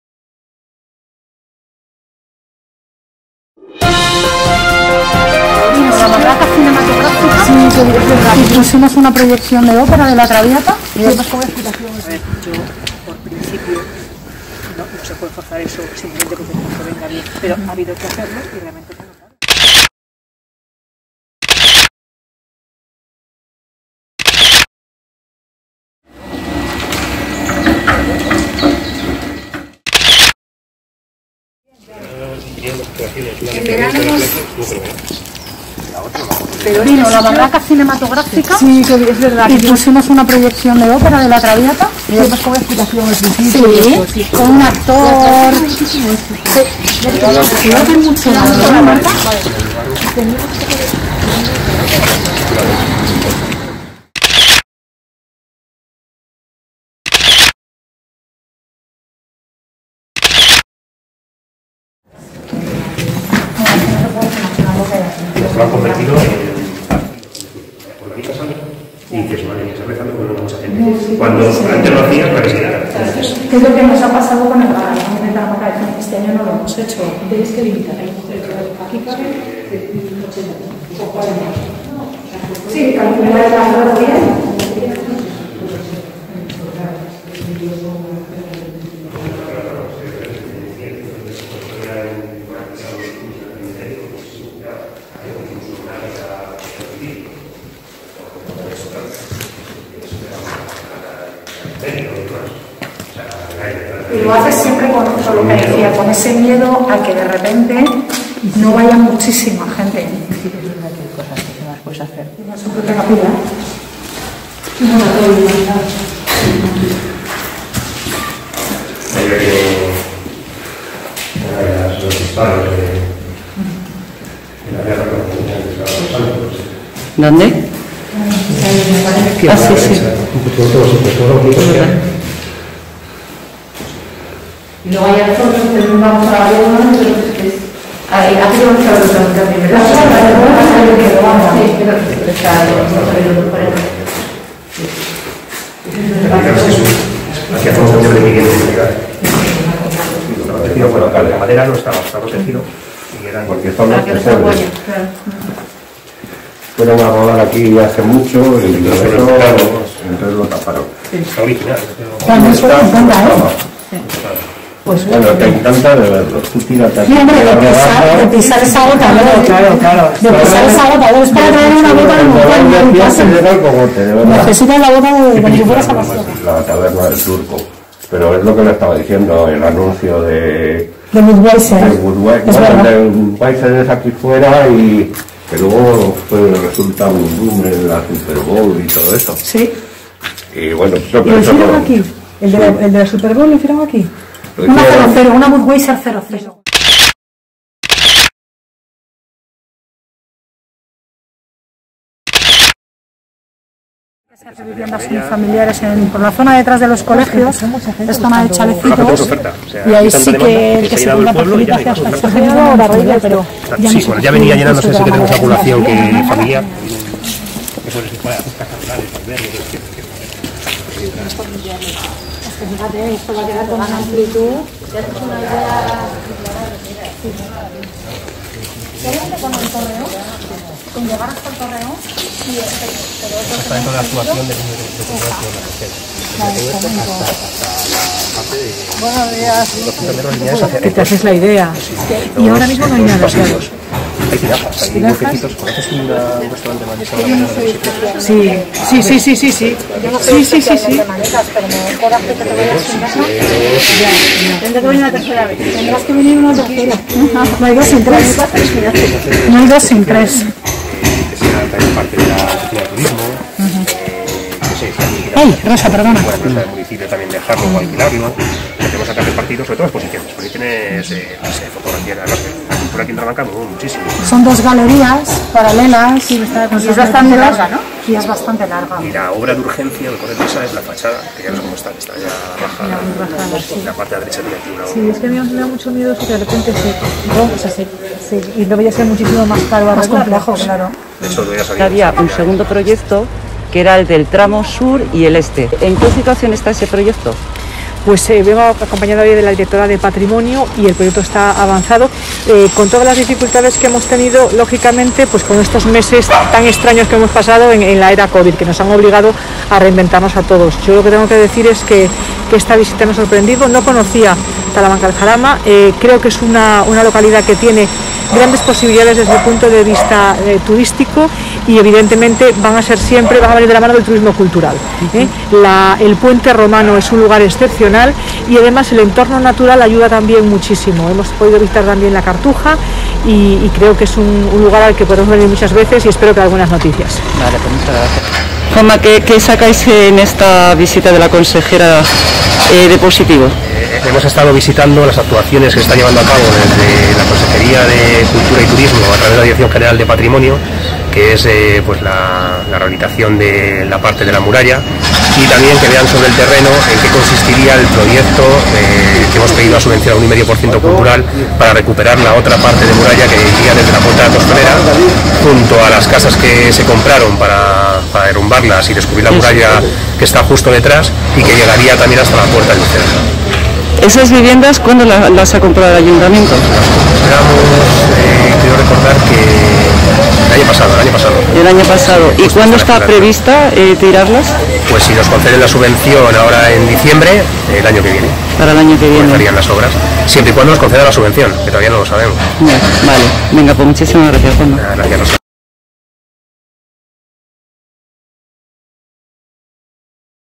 La consejera de Cultura y Turismo visita Talamanca de Jarama y sus obras de la muralla
La consejera de Cultura y Turismo de la Comunidad de Madrid, Marta Rivera de la Cruz, ha visitado el emblemático e histórico municipio de Talamanca de Jarama para ver, de primera mano, las obras de la muralla y la tapia de La Cartuja.

En el marco del compromiso179, Rivera de la Cruz, junto a la directora general de Patrimonio Cultural de la Comunidad de Madrid, Elena Hernando, fueron recibidas por el alcalde de Talamanca de Jarama, Juan Manuel García Sacristán, y la primer teniente de alcalde, María Moro.

Hicieron un recorrido por el municipio para ver las obras de consolidación del tramo norte de la muralla y la tapia de La Cartuja, el Puente Romano, así como también el exterior de la Iglesia de San Juan Bautista, el Morabito, la Puerta de la Tostonera, la Bodega del Arrabal y el Salón del Puente.